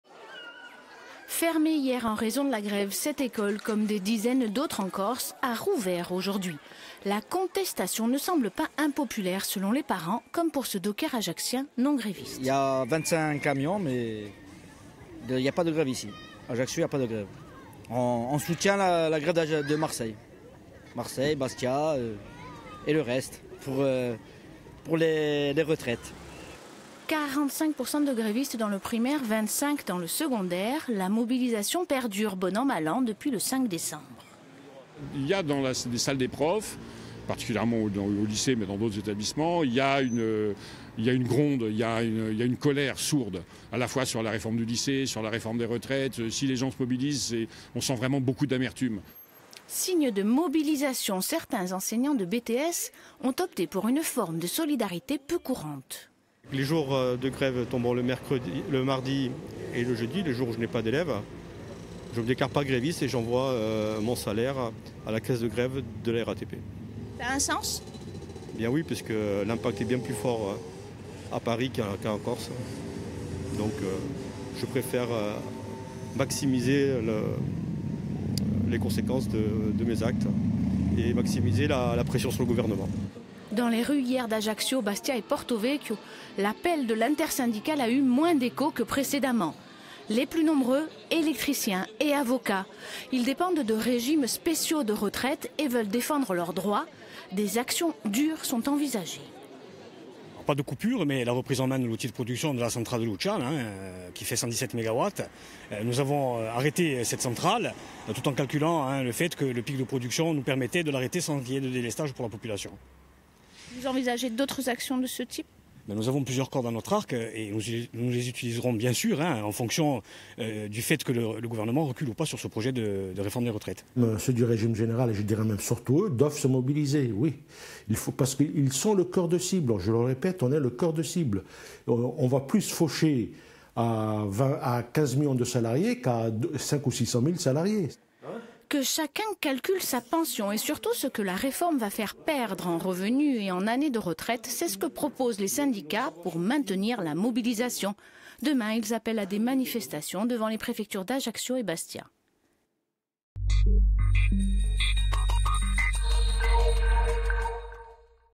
Corse : à quoi s'attendre après la grève nationale du 9 janvier ?
En Corse les effectifs des manifestations s'amenuisent mais le mouvement perdure. Jeudi 9 janvier se déroulait la quatrième journée nationale de grève . A Ajaccio près de 600 personnes sont descendues dans la rue . Certains manifestant suivent la cause depuis le début d'autres ont défilé pour la première fois c'est le cas des avocats du barreau d'Ajaccio.

Intervenants - Christian De Muro, docker non gréviste// Jean-Marc Puponi, secrétaire académique FSU SNES // Dominique Urbani, enseignant gréviste // Xavier Nesa, CGT EDF // Marcel Santini, secrétaire UD FO

Equipe - Marie-France Giuliani / Stéphane Agostini